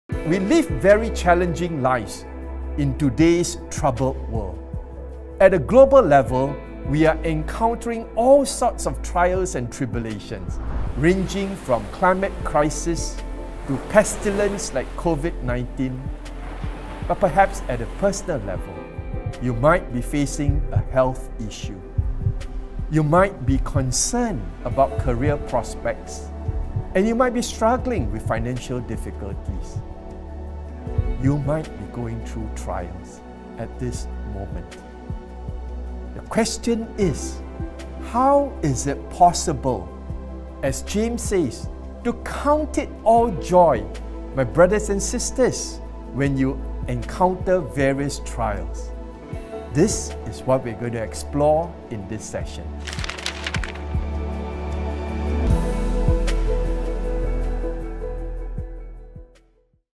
LIVING WISELY - REAL FAITH IN THE REAL WORLD [ER]
Learn from the Epistle of James "the Proverbs of the New Testament" how to be joyful in trials, and to live wisely.

LIVING WISELY - REAL FAITH IN THE REAL WORLD is a study series on the Book of James, led by Dr Ho Boon Tiong, Consultant Educationist.

Free access by registration.

Eagles Rendezvous is a ministry of Eagles Communications